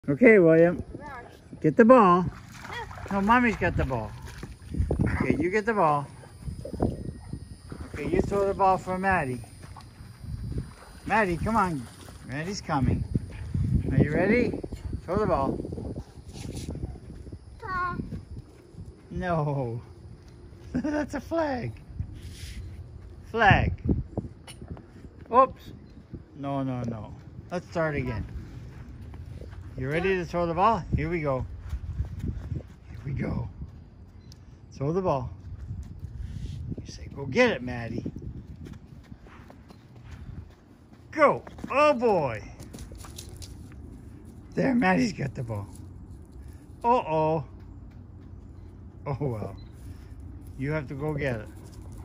Funny Great Dane & Grandpa Try to Teach Toddler How to Fetch A Ball
Maddie is enjoying the fact that Grandson Will is getting to the age where she can teach him dog stuff.  Watch & laugh as Coach Grandpa calls the plays, Maddie tries to teach Will how to fetch the ball .  It was a good first attempt, but there are going to be a few more spring training practices required.